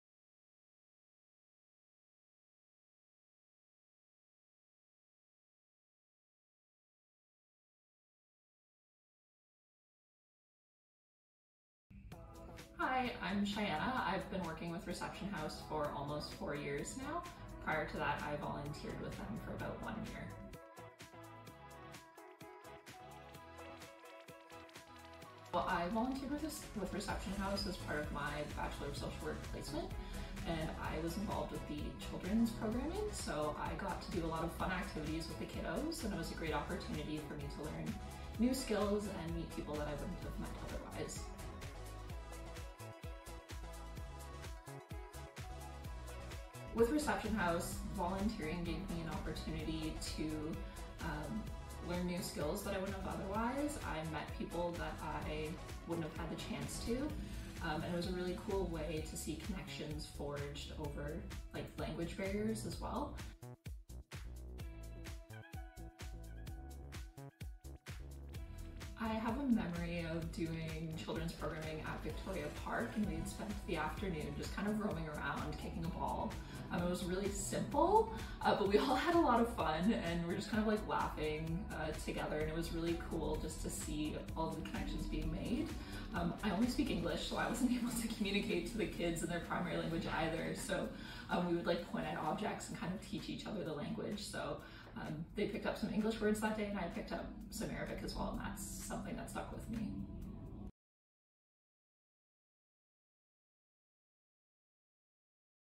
Meet Shianna at Reception House
I am excited to introduce you to our former amazing volunteer, "Shianna".
Shianna was a dedicated volunteer with Reception House before joining our staff. Let's hear from Shianna herself about her experience volunteering with us!